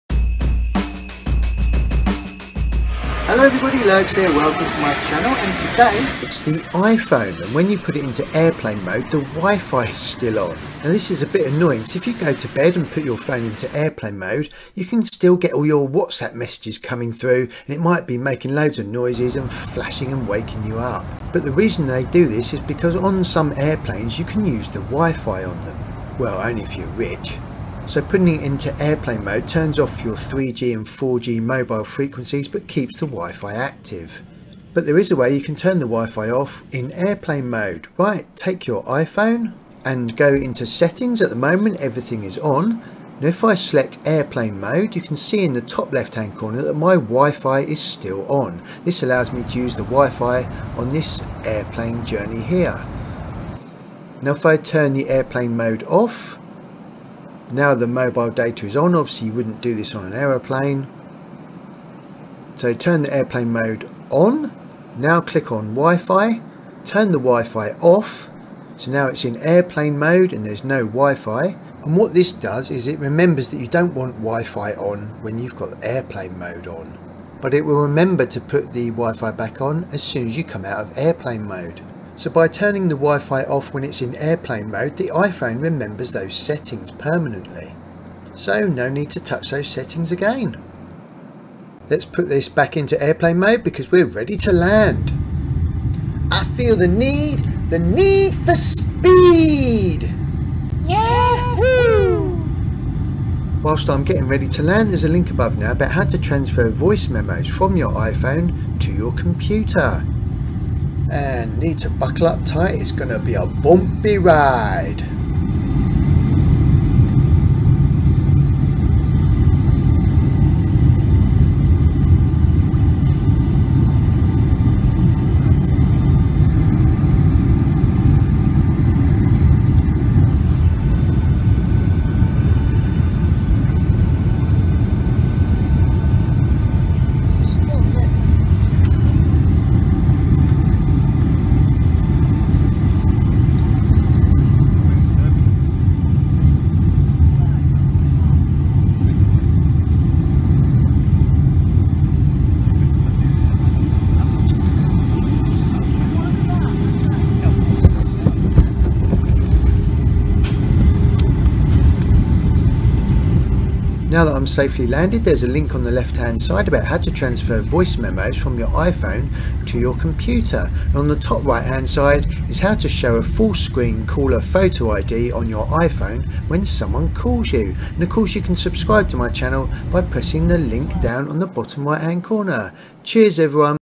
iPhone Airplane mode WiFi still On - WHY iPhone Airplane mode not turning off WiFi and how to FIX it
In this iPhone video I will show you how to turn off the WiFi when you put it into iPhone Airplane mode. It can be annoying if you are at home and you go to bed and put your iPhone into Airplane mode but the WiFi is still active so you still get pinged with WhatsApp and Instagram notifications!
There is a reason why you can have WiFi in Airplane mode and I will explain this.

Other useful How To Guides to help you save ⏱TIME and 💰 MONEY:
TECHNOLOGY